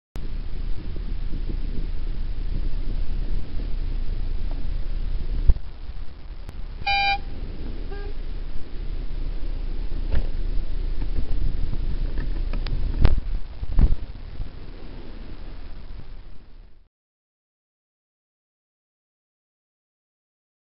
DU51 GXG
DU51 GXG was apparently never taught that we drive on the left in the UK.

DU51GXG DU51-GXG